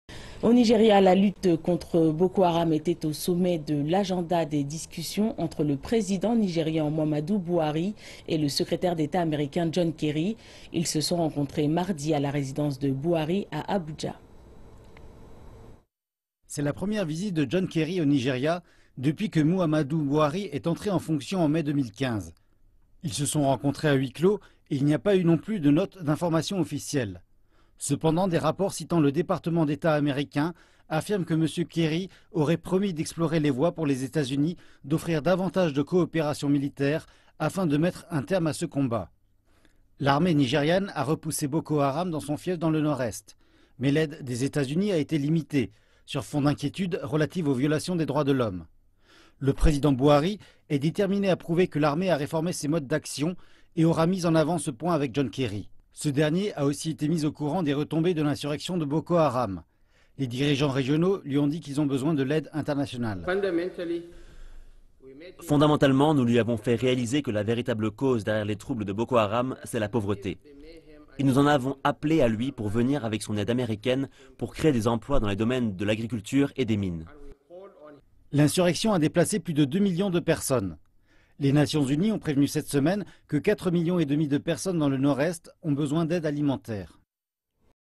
Les États-Unis promettent plus d’aide militaire pour combattre Boko Haram
Au Nigeria, la lutte contre Boko Haram était au sommet de l'agenda des discussions entre le président nigérian Muhammadu Buhari et le secrétaire d'État américain John Kerry.